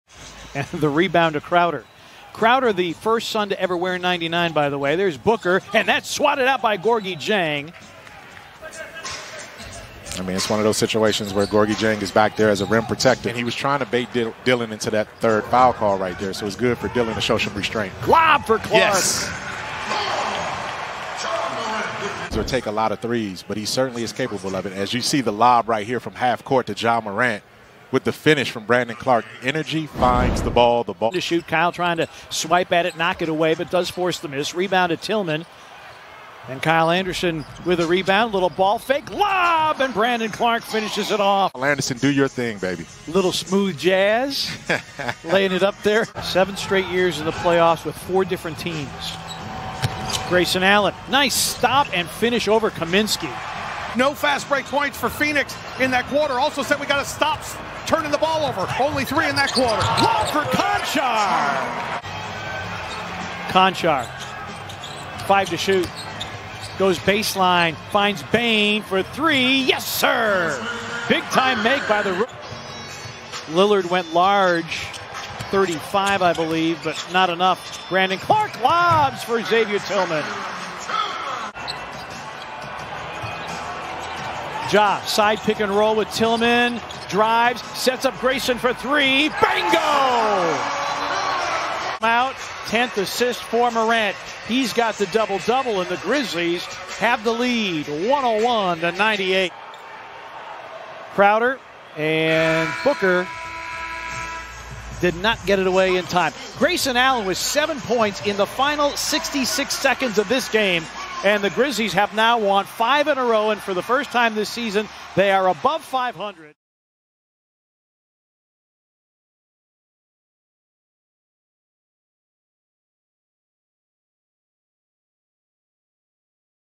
Memphis Grizzlies vs Phoenix Suns Full Team Highlights | January 18, 2021 | NBA Season 2020-21
See full team highlights from the Memphis Grizzlies fifth consecutive win beating the Phoenix Suns 108-104 while dawning their 20th season edition jerseys on MLK Day in FedExForum on Monday, January 18, 2021.